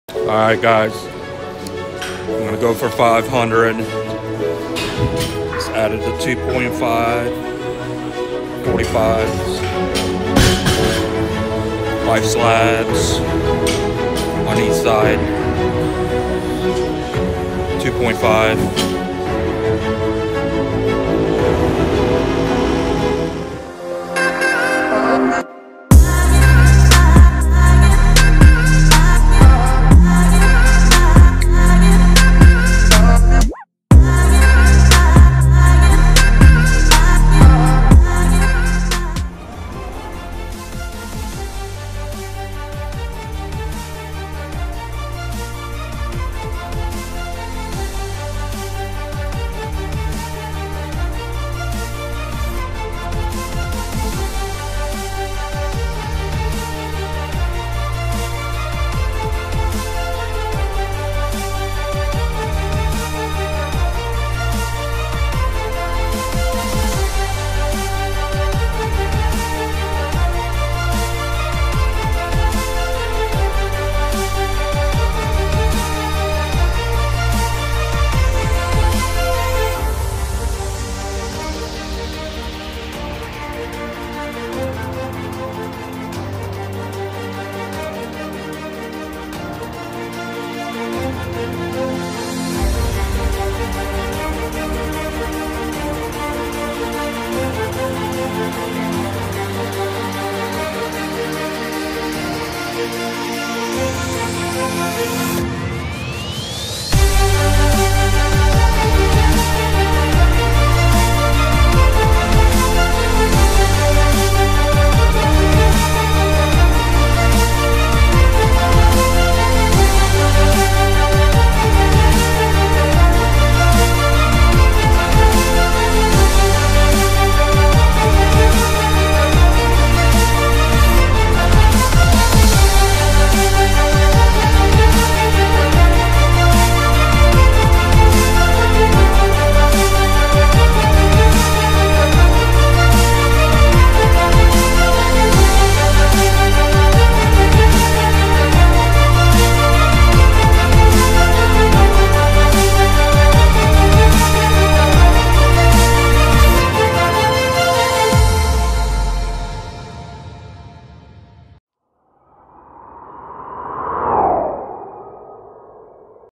500 LBS Back Squat GOAL ACHIEVED!! 2 YEAR TRANSFORMATION from 185 lbs to 500 lbs Back Squat
It’s been a journey guys. I remember when I could barely squat 185 lbs 2 years ago. Never in a million years did I EVER think I would squat 500 lbs. I’m SUPER excited that I achieved my goal. It’s hard to see the 2.5 lb plates on the last clip but it was there lol! Yes, it was HEAVY as F$$K!! I just kept my core tight as possible throughout the whole lift. If you guys ever notice, I NEVER use a safety bar on the Rack because I want to feel like I HAVE to make the lift.